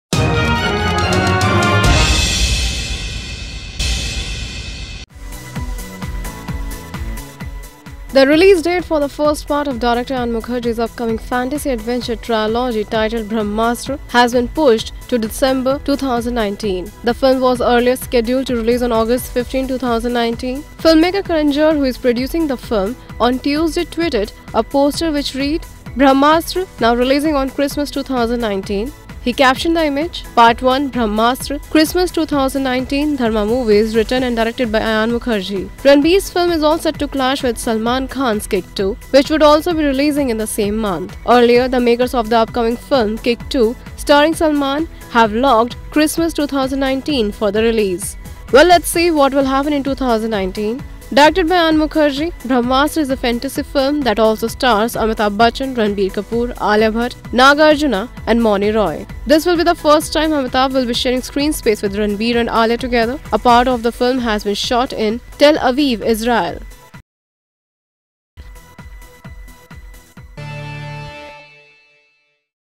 Ranbir vs Salman in 2019? BRAHMASTRA to clash with KICK 2
The release date for the first part of director Ayan Mukerji's upcoming fantasy adventure trilogy titled "Brahmastra" has been pushed to December 2019.  Ranbir’s film is all set to clash with Salman Khan’s Kick 2, which would also be releasing in the same month.  Directed by Ayan amplifying. "Brahmastra" is a fantasy film that also stars Amitabh Bachchan, Ranbir Kapoor, Alia Bhatt, Akkineni Nagarjunaa and Mouni Roy.